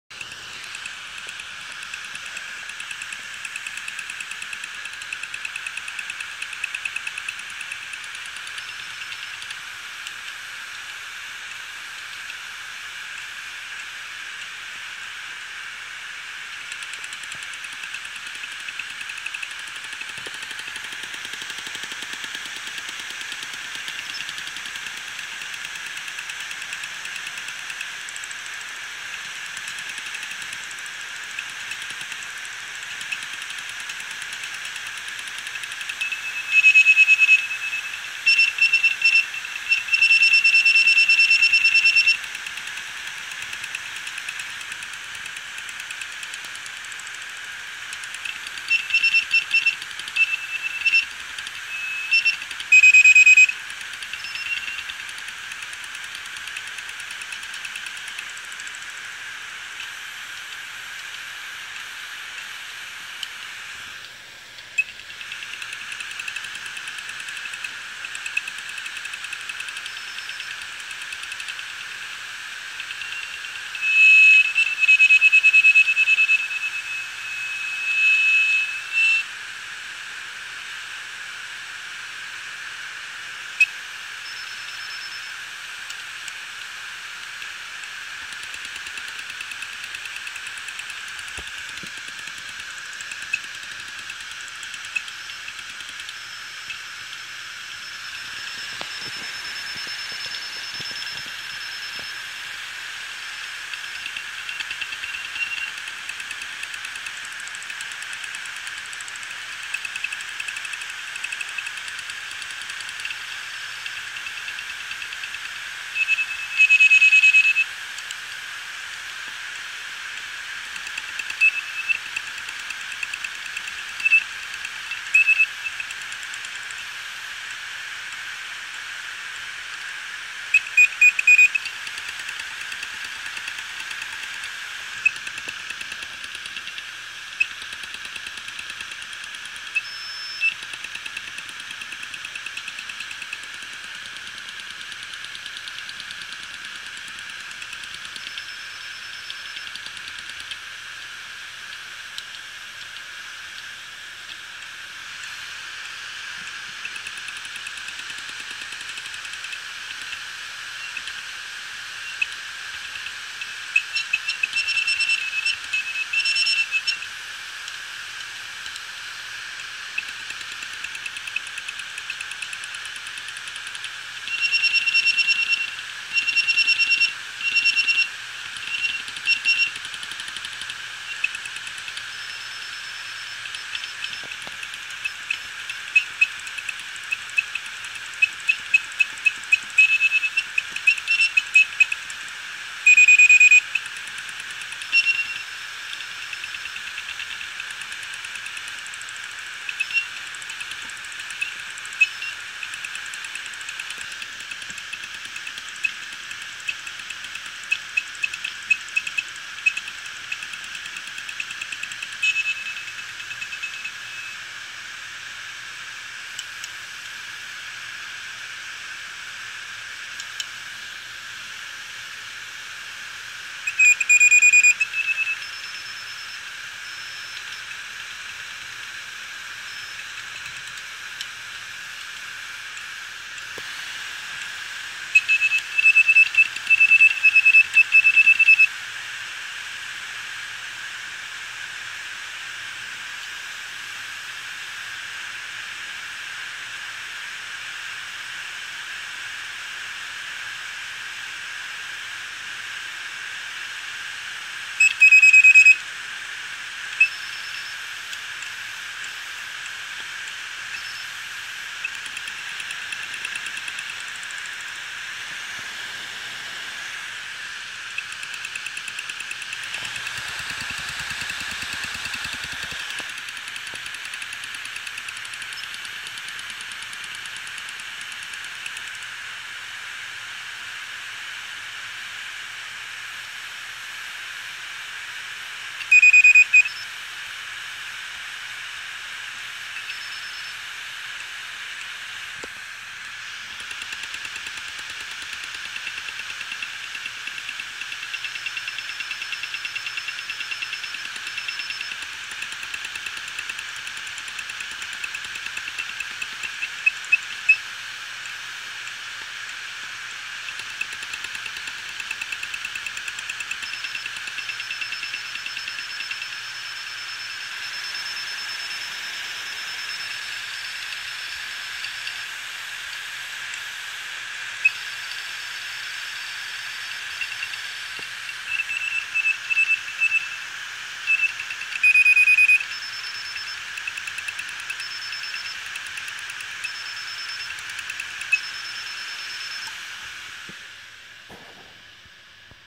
hard drive making funny noises
an old seagate 7200.11 that doesnt like the cover off. this specific one was already dead from the sd15 firmware issue.